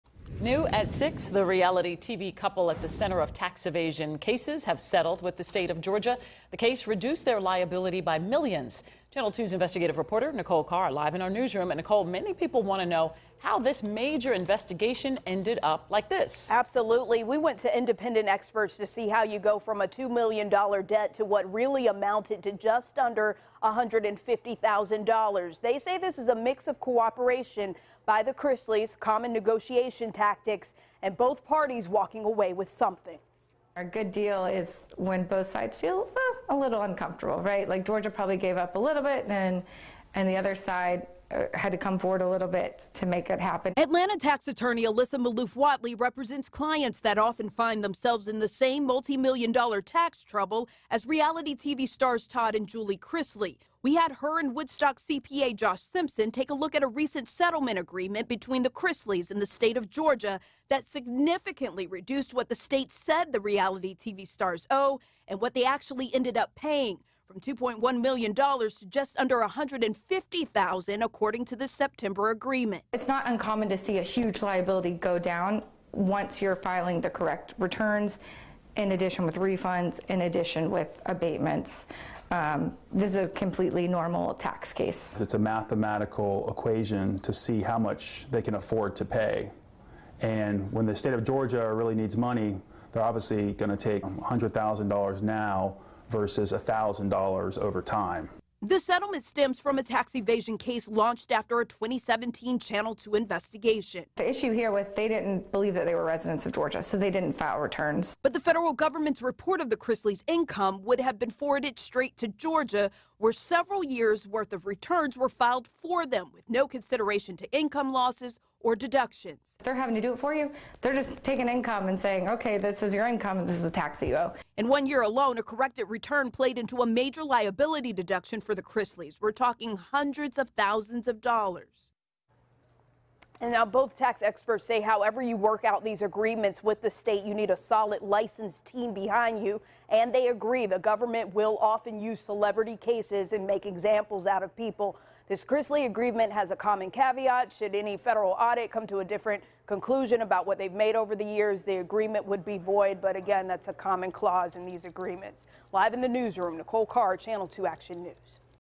State settles Chrisleys' $2.1 million tax evasion case -- but how? | WSB-TV
The Georgia Department of Revenue has settled its $2.1 million tax evasion charge against Todd and Julie Chrisley, of "Chrisley Knows Best" fame. Over a span of eight years from 2008 to 2016, the Chrisleys were found to have overpaid taxes for four of those years, owed nothing for four more and underpaid $77,000 one year, according to a press release from the Chrisley's public relations firm. That amount has been paid, it said.

We talked to tax experts to break down how the agreement was made.

CONNECT WITH WSB-TV:
Like WSB-TV Channel 2 Action News on FACEBOOK Follow WSB-TV Channel 2 Action News on TWITTER